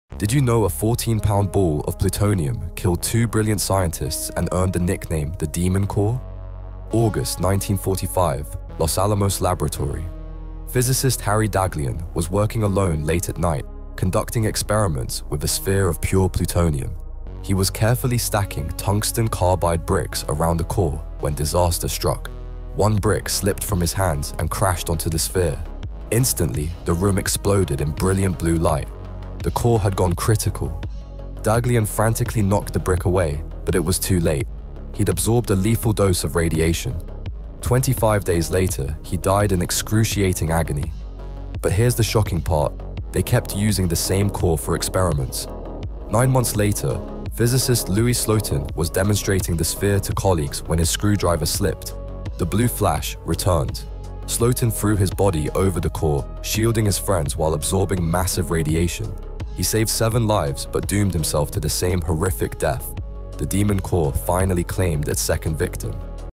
The Demon Core: How a 14lb Plutonium Ball Killed Two Brilliant Scientists
This radioactive sphere killed 2 scientists 😱